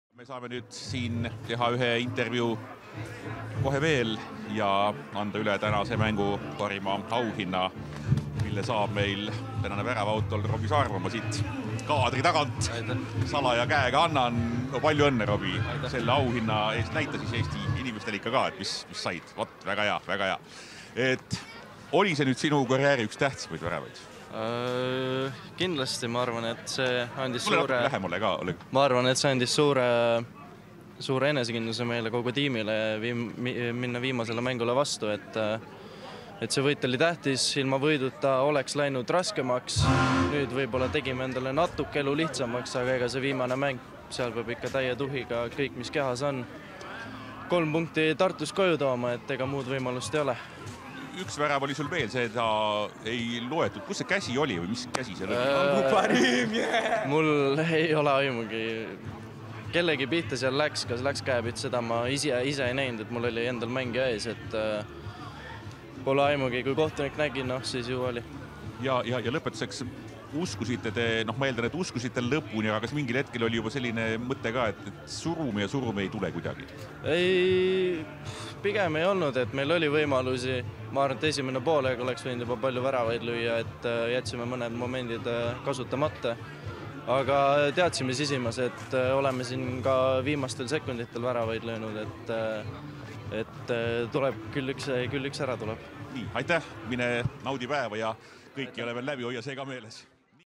Paide Linnameeskond - Nõmme Kalju FC l 1:0 l Premium liiga 35. voor l 2022 l Robi Saarma
6. novembril kell 17.15 Paide kunstmurustaadionil

Paide Linnameeskond - Nõmme Kalju FC 1:0 (0:0)
75. Robi Saarma